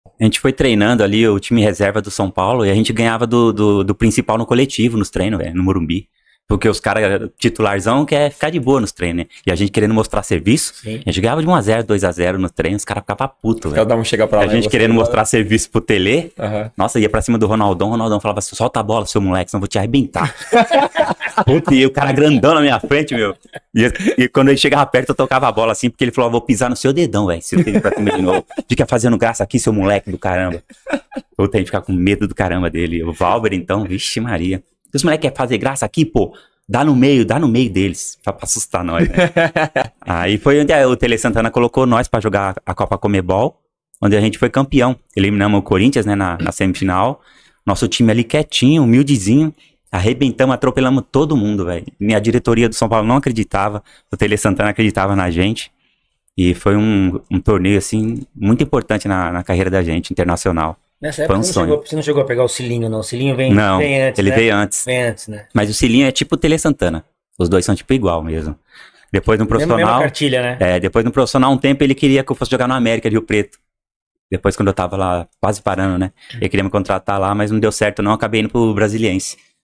Time Reserva ganhava do Time Titular do São Paulo!
Time reserva recheado de jogadores da base do tricolor ganhavam coletivo do time dos profissionais na época do Telê!😬 Esse time que era chamado de "Expressinho" dava trabalho na época em!?

Podcast completo com Pavão já está disponível no YouTube e Spotify 📲⚽ Link na Bio! Imperdível!